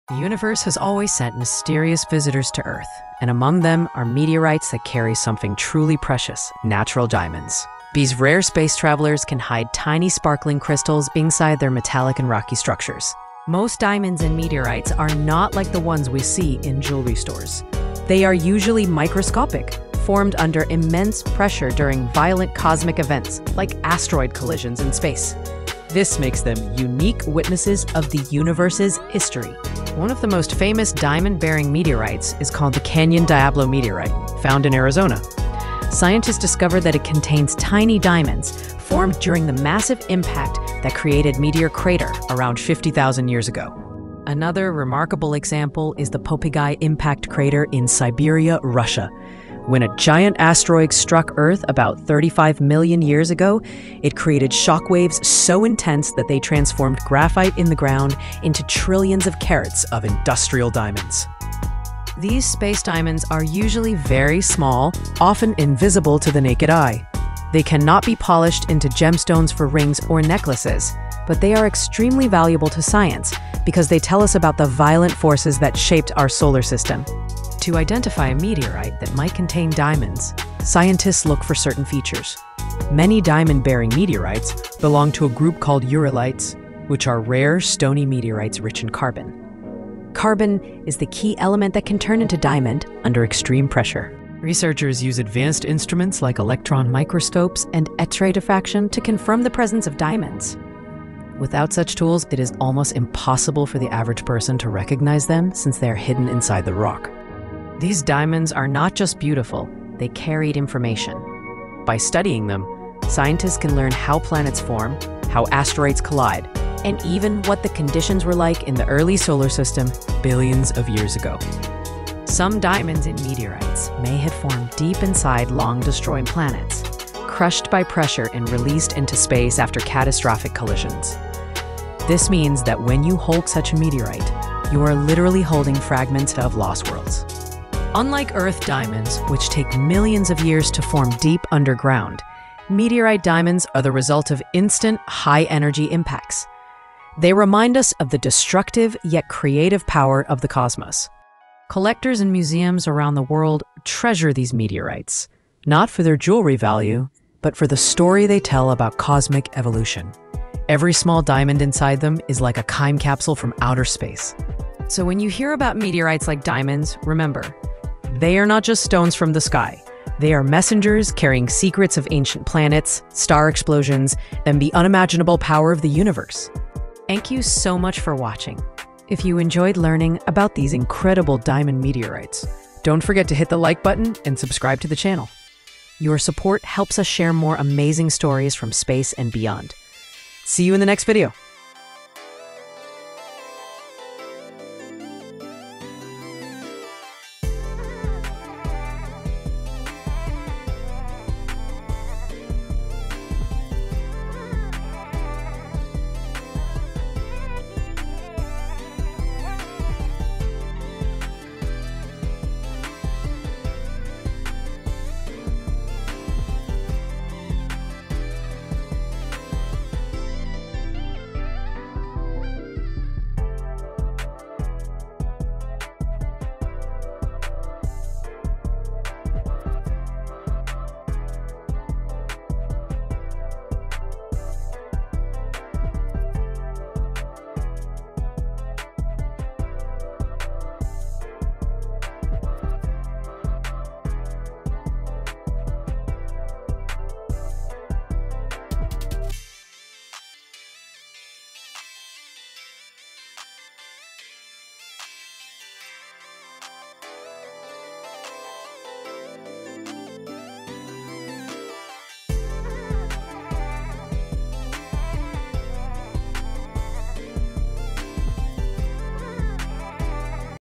Meteorites with Diamonds: Secrets from Space 🌌💎
Did you know some meteorites contain real diamonds? 💎✨ In this video, we explore the most famous diamond-bearing meteorites, where they come from, and how scientists identify them. Discover the secrets of Canyon Diablo, Popigai Crater, and the mysterious ureilites that carry diamonds from outer space. These tiny gems are not just beautiful—they hold the story of lost planets and cosmic collisions. 🌍🚀